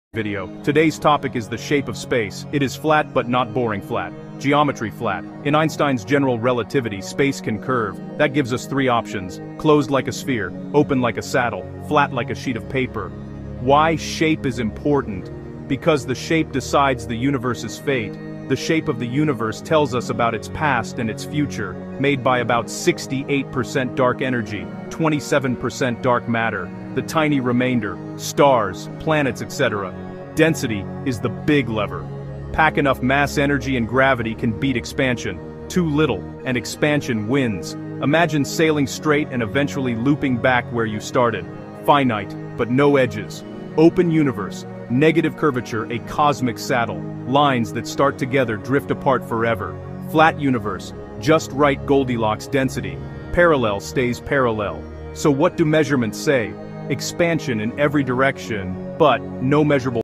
The Shape of Universe: Theorical Assumptions
Explore the shape of the cosmos in 2 minutes: flat, open, or closed? This fast explainer uses stock footage to visualize how general relativity lets space curve like a sheet, saddle, or sphere — and why that curvature determines the universe’s fate. Learn how density, dark matter, and dark energy influence expansion, what “flat” really means for the observable universe, and simple rocket-path examples that make curvature intuitive. Perfect for curious minds who want big ideas explained clearly and quickly. Keywords: universe shape, flat universe, open universe, closed universe, curvature, cosmology, dark energy, observable universe.